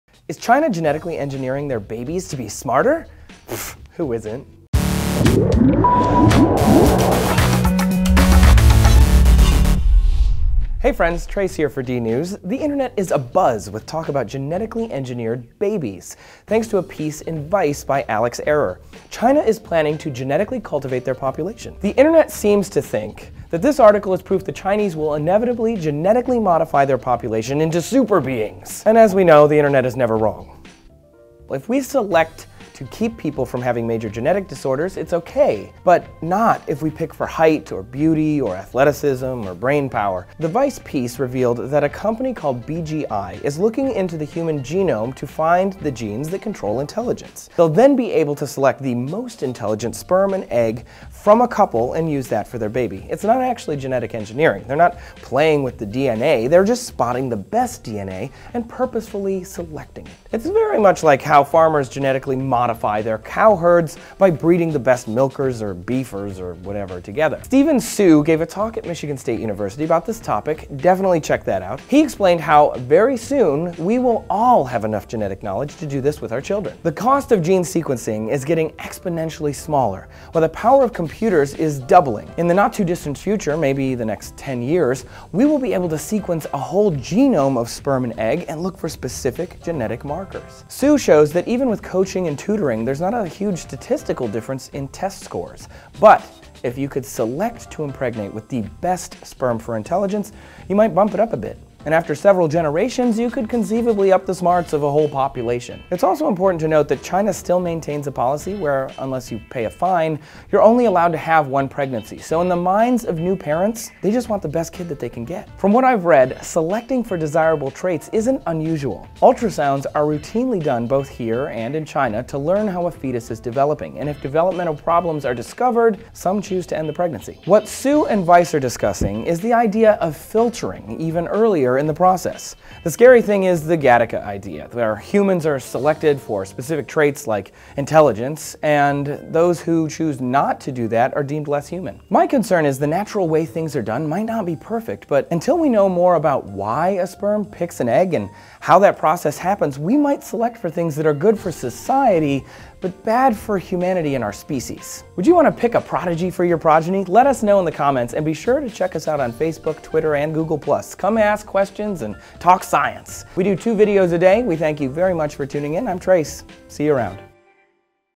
Is China Engineering Smarter Babies?
One of the most advanced genomics institutes in the world is working hard to create a new generation of super-smart kids. So how smart are we talking, and what might this mean for future generations? Trace uses his own super powerful brain and finds out.

"CHINA IS ENGINEERING GENIUS BABIES"

"At BGI Shenzhen, scientists have collected DNA samples from 2,000 of the world's smartest people and are sequencing their entire genomes in an attempt to identify the alleles which determine human intelligence."

"Genetic Architecture of Intelligence"

"Seminar at Michigan State University Cognitive Science Forum, 2/15/13."

"BGI Cognitive Genomics"

BGI Cognitive Genomics is an interdisciplinary research group at BGI, one of the largest genomics institutes in the world. Our focus is human cognition, with emphasis on the use of tools made available by rapid advances in DNA sequencing technology.

"On the genetic architecture of intelligence and other quantitative traits"

"Those genius Chinese babies"

"I've gotten several emails about the Vice interview of Geoffrey Miller on BGI's Cognitive Genetics Project."